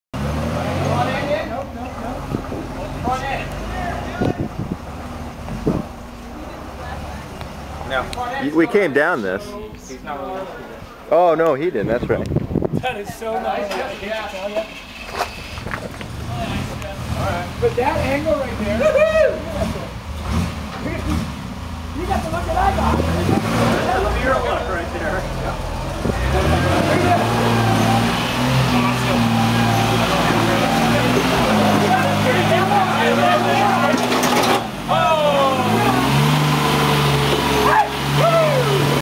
2008 Calico Mogfest Unimog
Kelly shows us how to squeeze through Odessa Canyon after a recent washout.